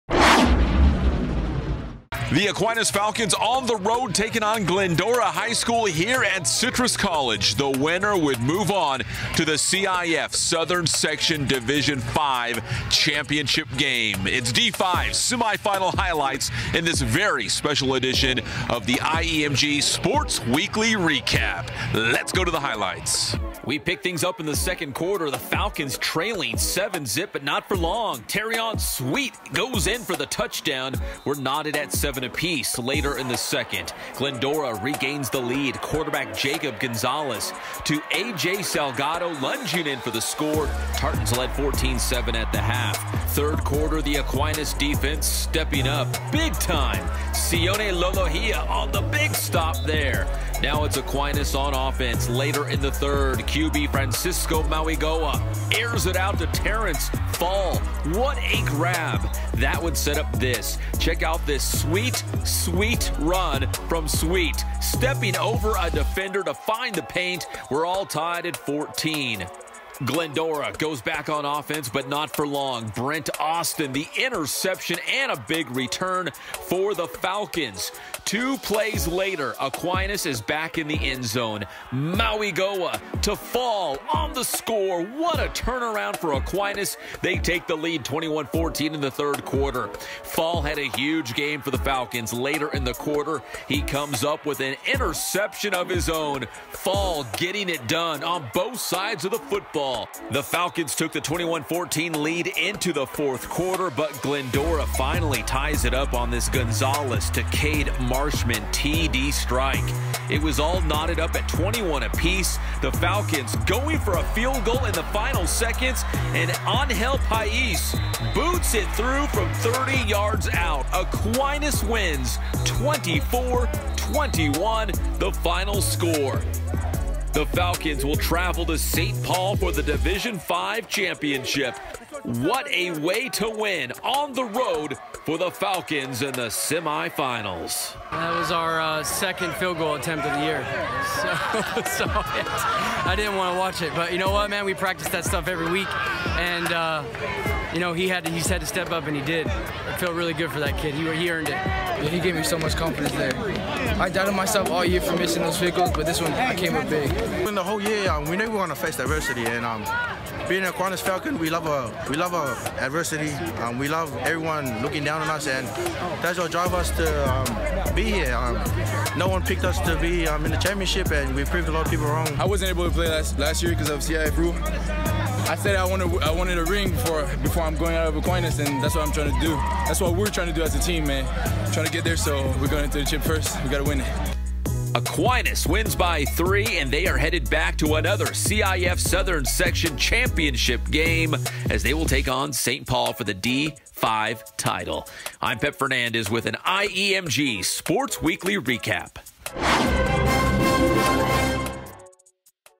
IEMG Sports Weekly FOOTBALL: Aquinas vs. Glendora Recap
(San Bernardino, CA) - The Aquinas Falcons win 24-21 to advance to the CIF Southern Section Division 5 championship game.